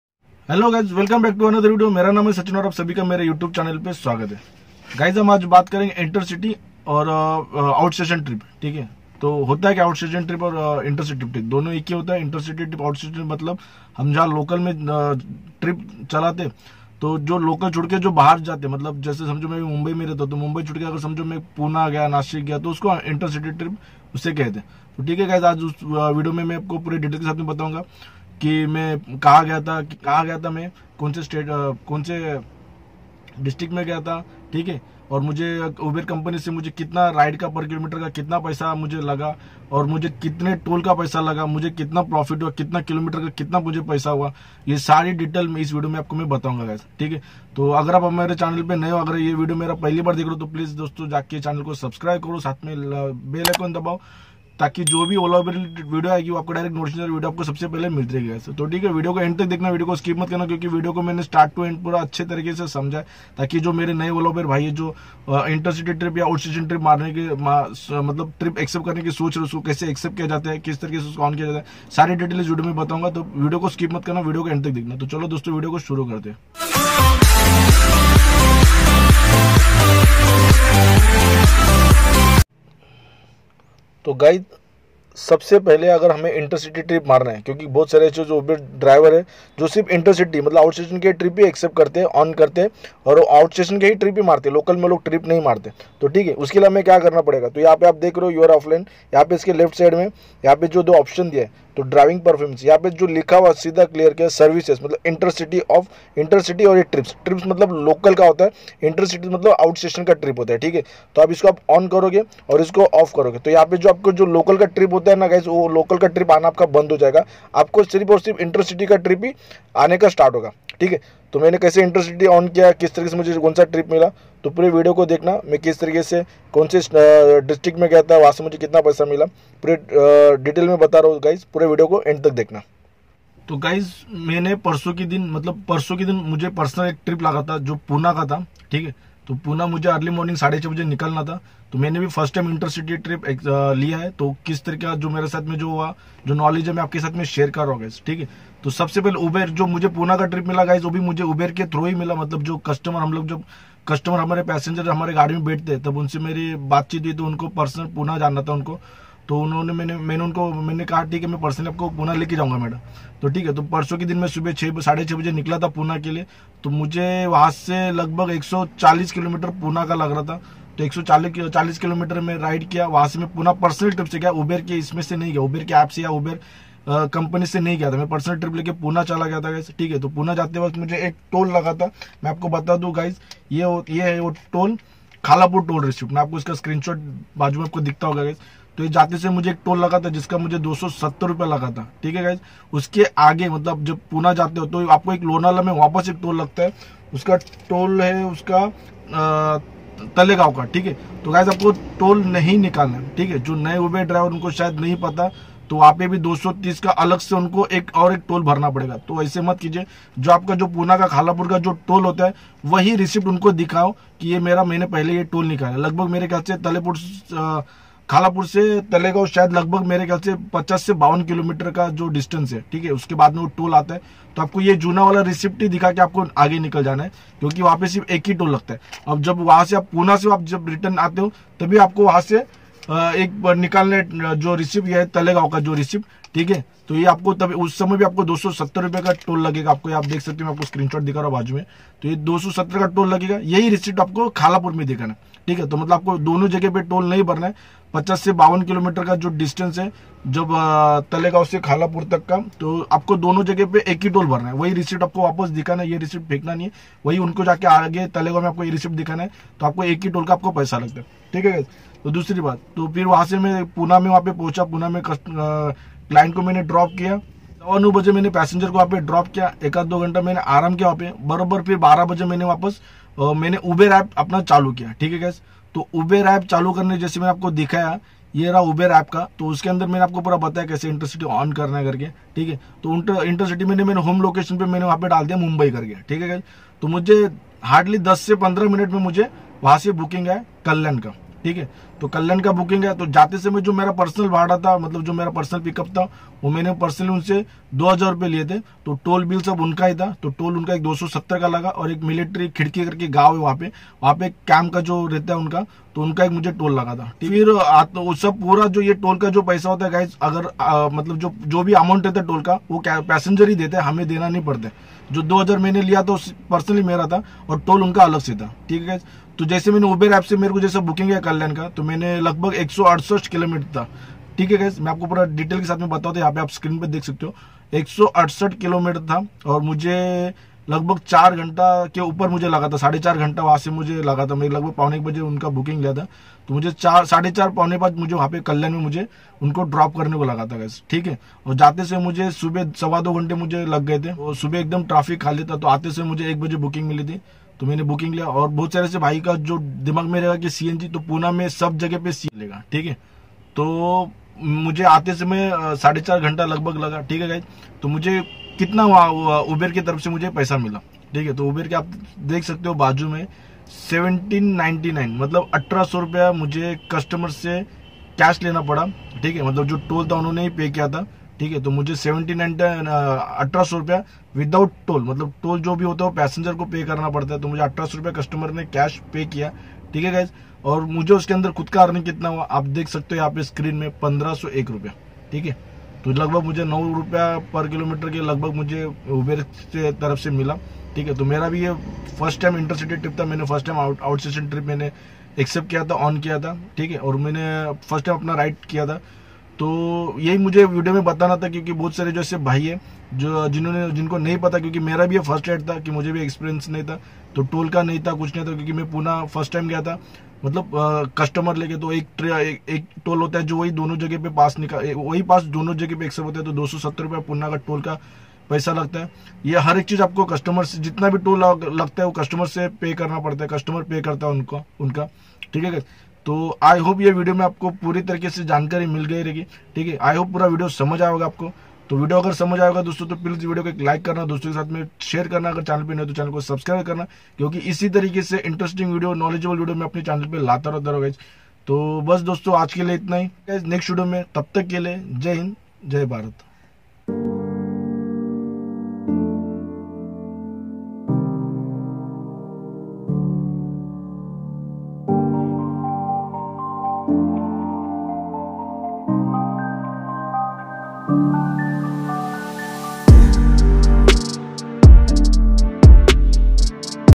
intercity trip/outstion trip कैसे acept करते है?? और kitna पैसा uber cab देता है।।👍
dosto es video me apko puri jankari mil jayegi kis tarah trip accept karte hai..kaise jate hai..kitna toll hame lagta hai.vidio ko puara jarur dekhna.puna trip.

MY GEARS👇

TRIPOD FOR MOBILE :-👉👇